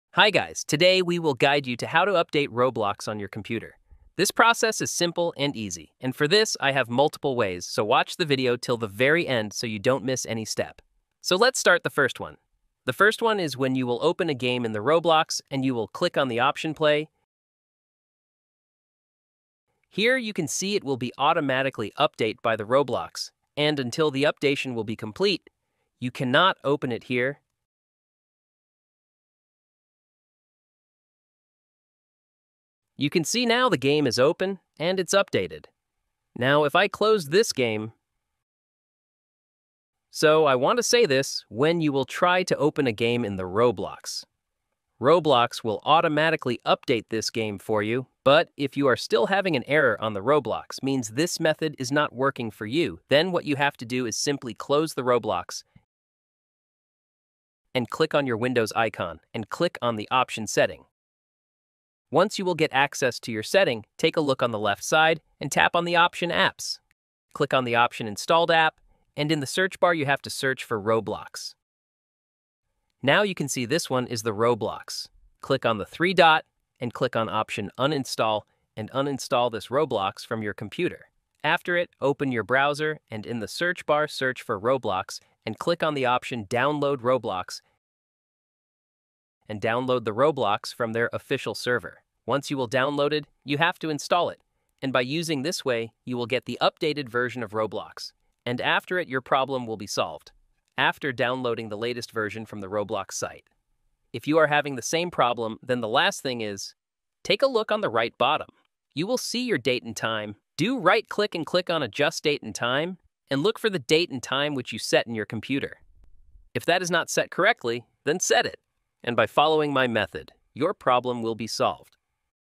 Update Your Roblox NOW to the LATEST VERSION on PC and Laptop! ✅
Learn how to update Roblox to the latest version on your PC or laptop! In this tutorial, we'll show you the step-by-step process to ensure you're running the most recent version of Roblox. Stay ahead of the game and avoid any issues by keeping your Roblox client up to date. Whether you're a seasoned player or just starting out, this video is perfect for anyone looking to update Roblox on their computer.